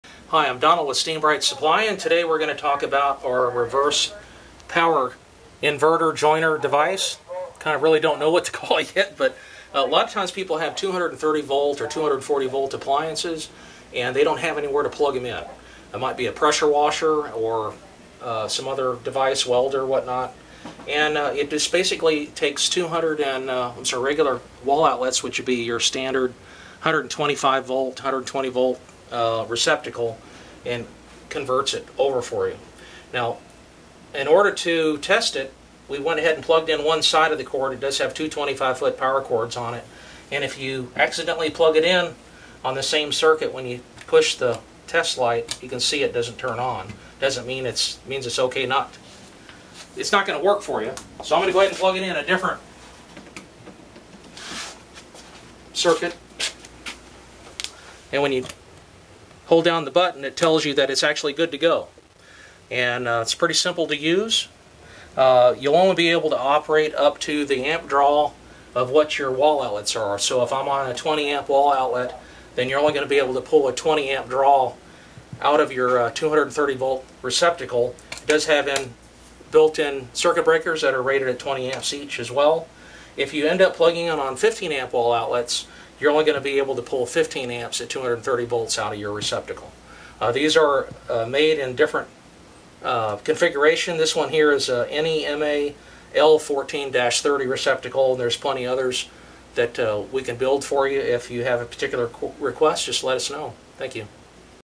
Steambrite: 230 Volt to 240 volt Circuit Joiner using two 120 Volt outlets
Having difficulty finding where to plug in you 230 and 240 volt electric car, appliance, pressure washer, vapor steam cleaner?  Charges electric cars 100% faster than standard 120 volt outlets. Used on Stopsaws, planers, and other industrial woodworking equipment.
No any more with this electrical converter box
120 to 240 volt conveter